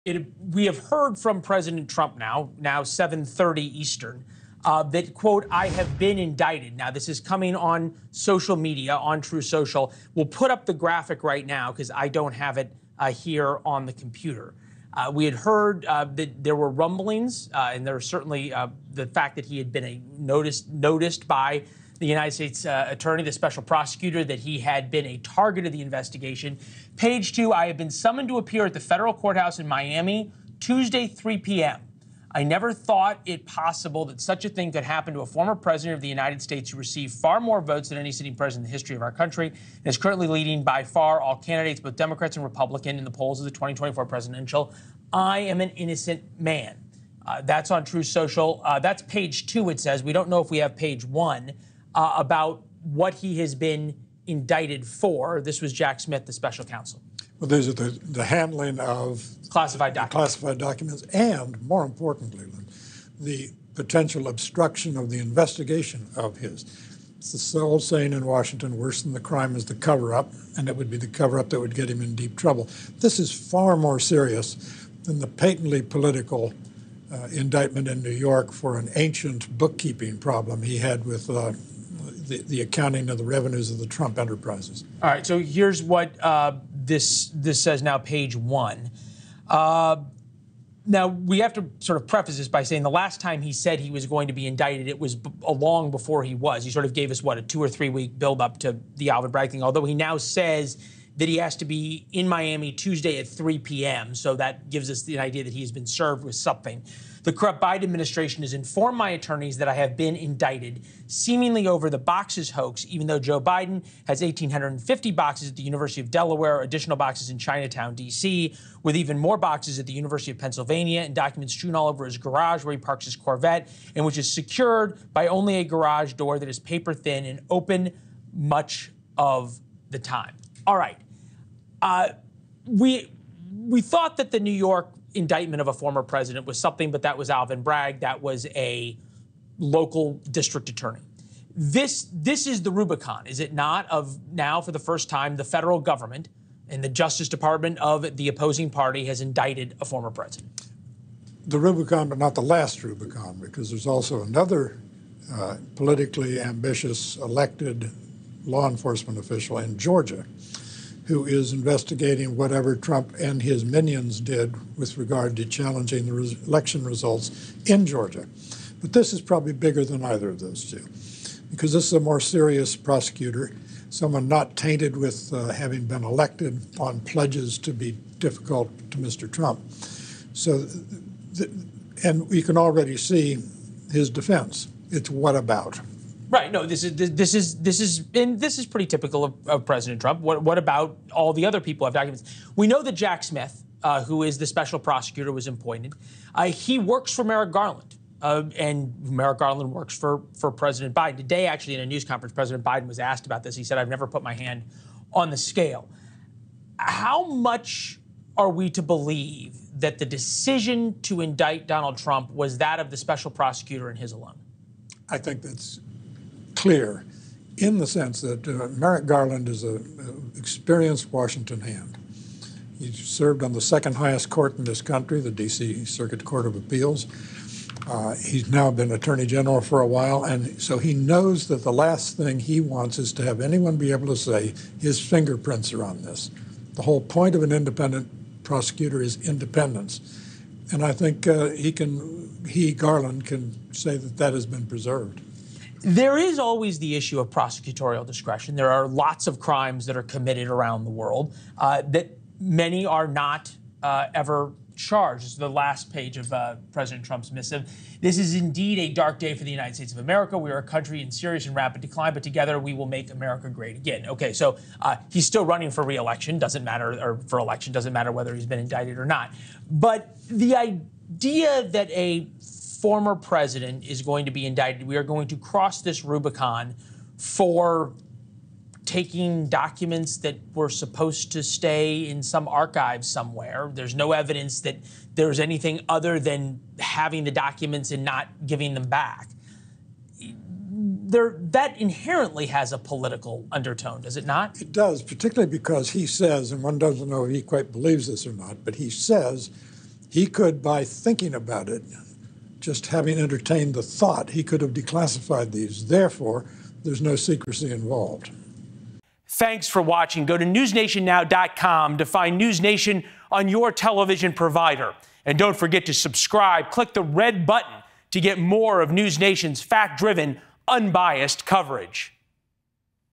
Trump indicted in classified documents case | On Balance
NewsNation sources confirmed that a grand jury has indicted former President Donald Trump following an investigation into his handling of classified documents.